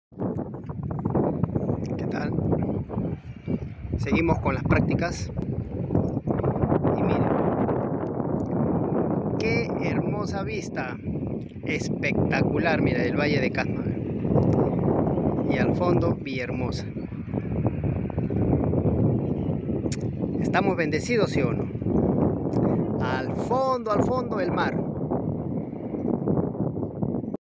CASMA Y VILLA HERMOSA
hola amigos aquí les presento a casma y a villa hermosa por dónde vivo